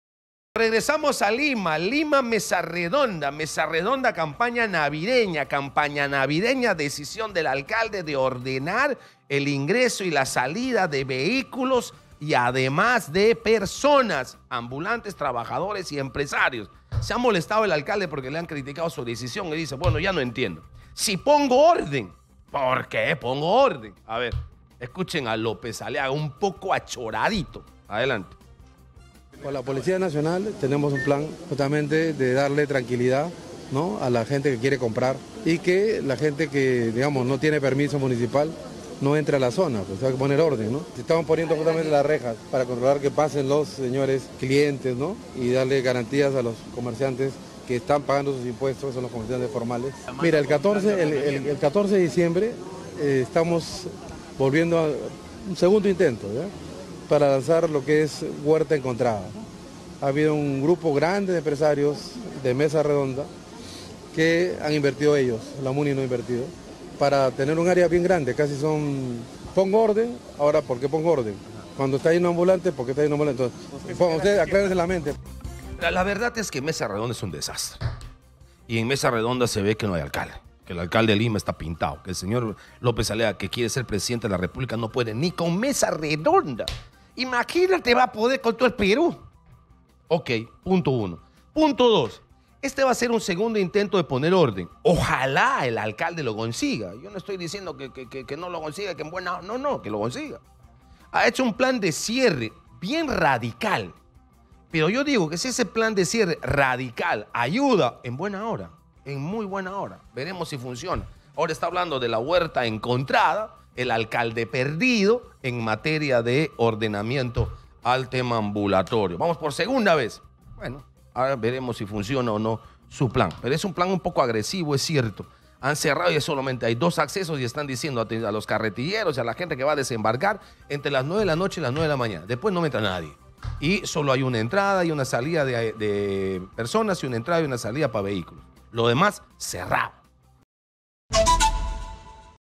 🔴🔵RLA responde a críticas por enrejado en Mesa Redonda: "Aclaren su mente"
El alcalde de Lima, Rafael López Aliaga, defendió la instalación de rejas en Mesa Redonda como parte de un plan integral de seguridad y orden, resaltando que la medida busca proteger la vida y el comercio formal.

Exitosa Perú
"Exitosa: La voz que integra al Perú"

Corporación Universal